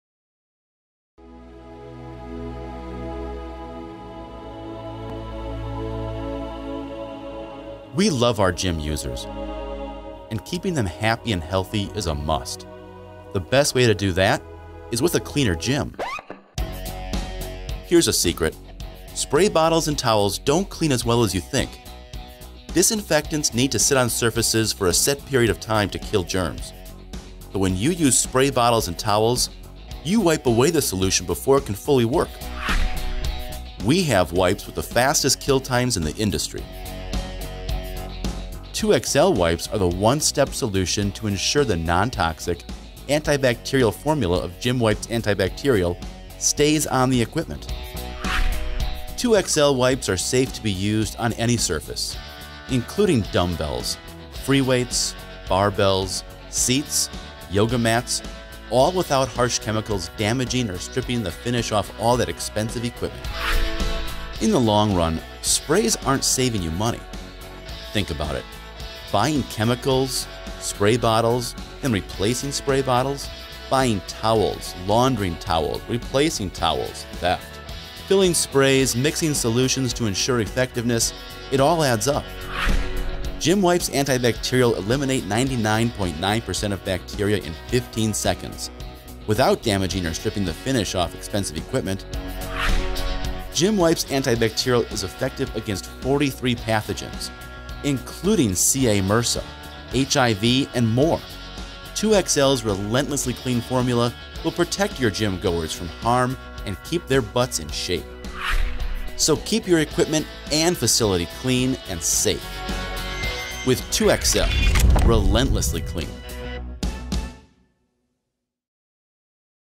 GymWipes – antibakterielle våtservietter
GymWipes er ORGINALEN og no1 på antibakterielle våtservietter. Dreper virus, som Covid-19 og bakterier på treningsutstyr og overflater